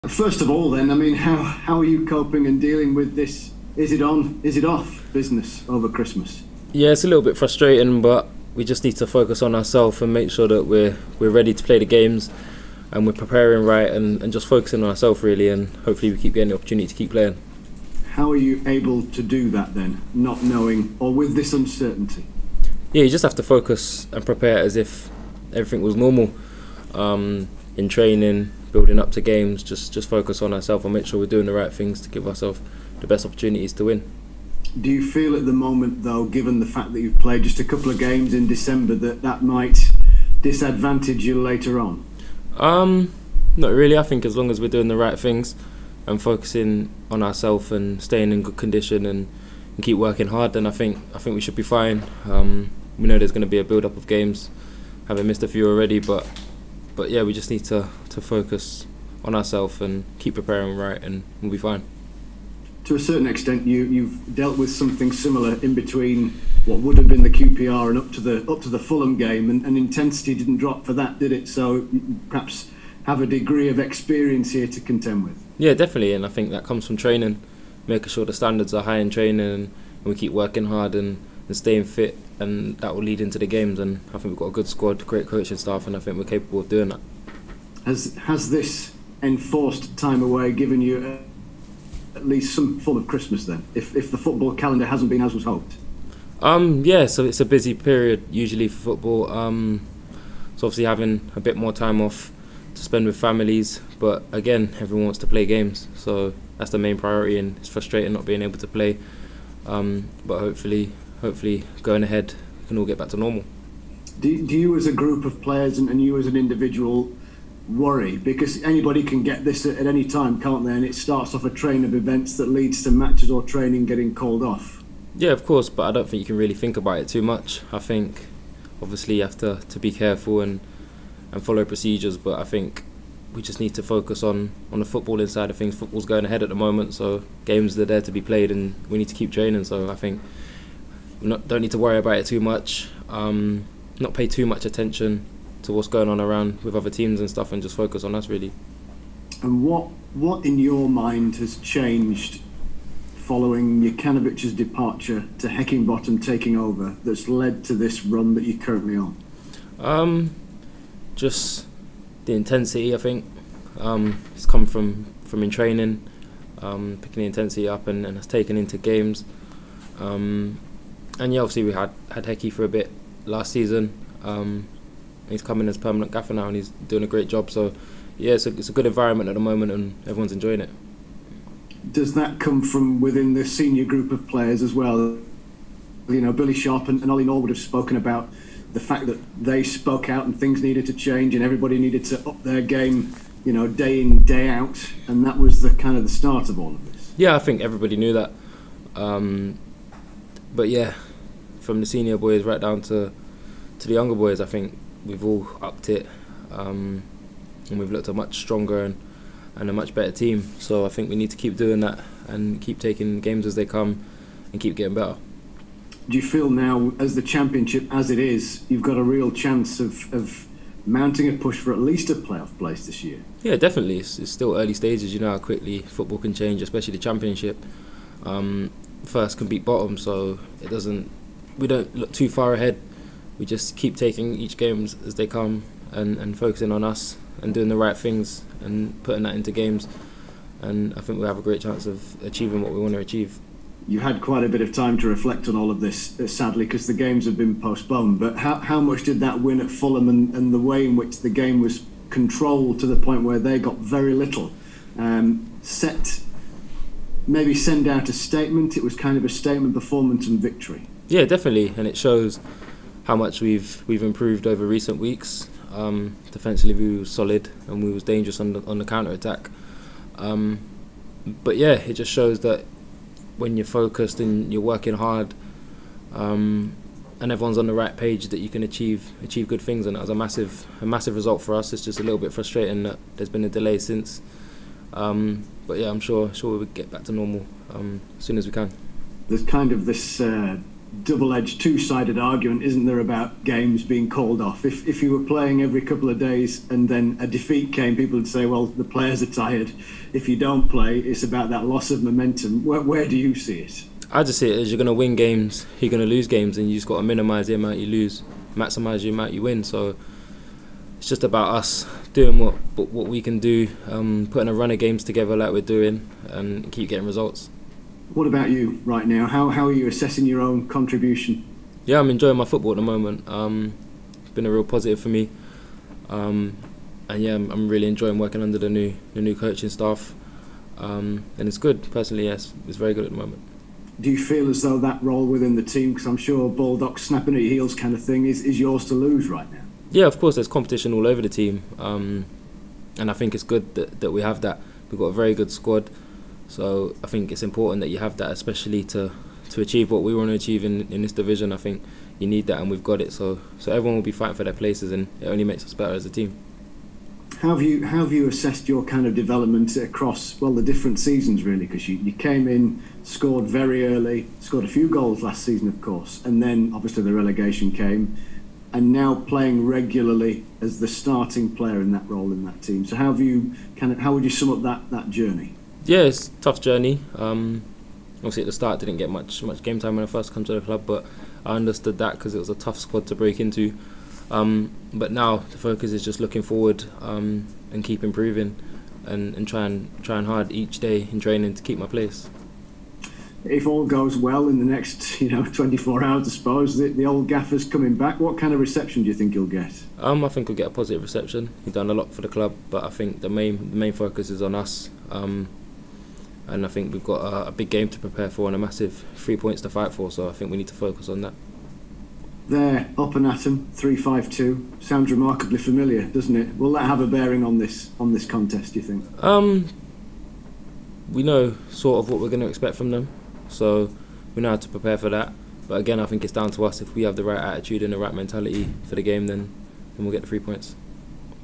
Jayden Bogle | Sheffield United v Middlesbrough | Pre-match press conference
Jayden Bogle looks ahead to Middlesbrough's New Year's Day visit to Bramall Lane.